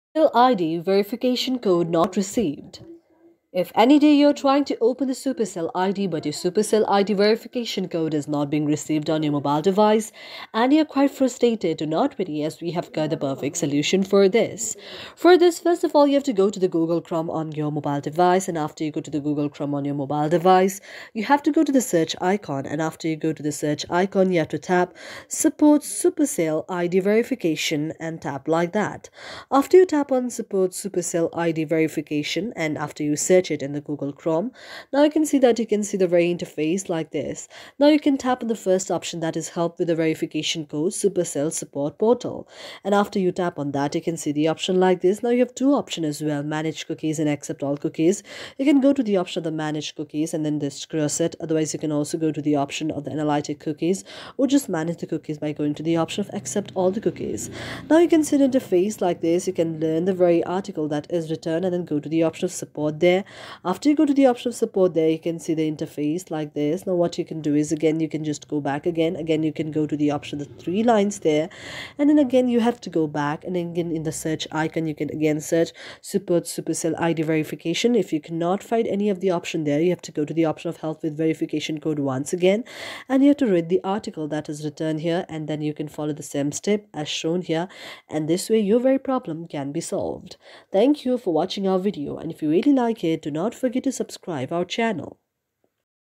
How To Fix Supercell ID Verification Code Not Received (WORKING!)
Encountering issues with receiving verification codes for your Supercell ID? You're not alone! In this comprehensive tutorial, we'll guide you through the step-by-step process of fixing the Supercell ID verification code not received problem, ensuring you can successfully verify your account and access your favorite games without any hassles.

In this tutorial:
0:00 - Introduction
0:15 - Supercell ID Verification Code Not Received & Not Coming Problem Solved | Gmail OTP Not Sent in COC

Join us as we explore various troubleshooting methods to resolve the Supercell ID verification code not received issue. Whether you're using Clash of Clans, Clash Royale, or any other Supercell game, we've got you covered! From checking spam folders to contacting support and using alternative verification methods, we'll cover everything you need to know to get back into the game.

Get ready to overcome the verification code issue and enjoy uninterrupted gameplay by watching our comprehensive tutorial today!